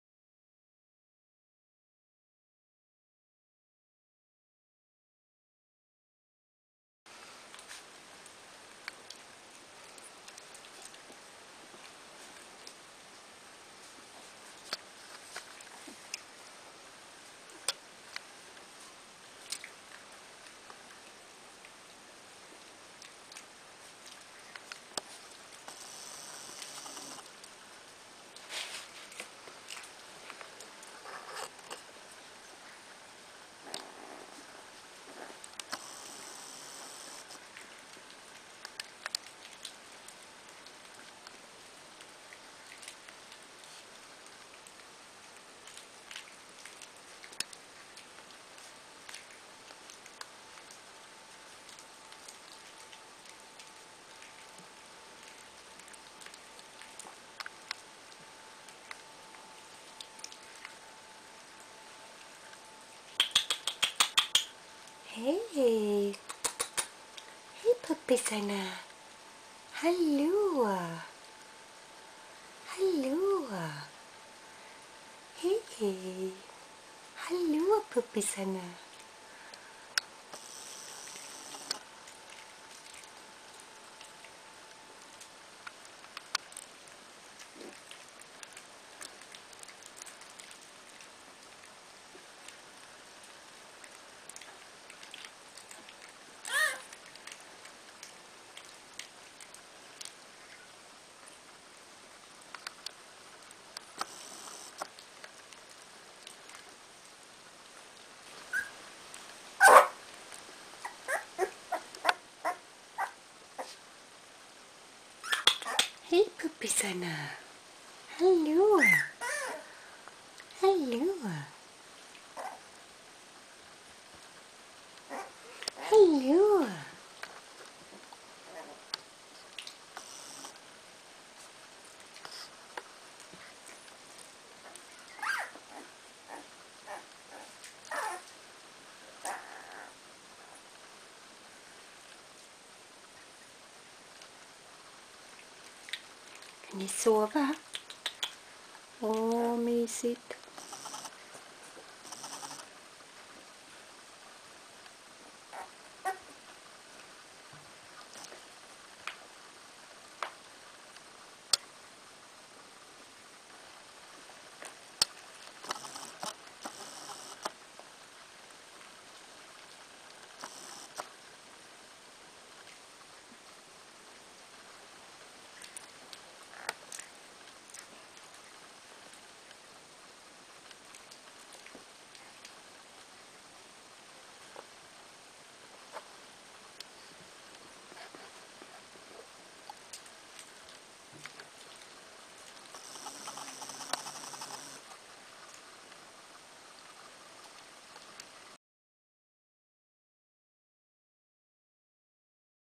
Star Trek pups listening to my voice
Now the puppies can see and hear and they have started to listen to sounds. They are also biting more:-) Star Trek Litter 18 days old. Welsh Corgi Cardigan at Fromax Kennel.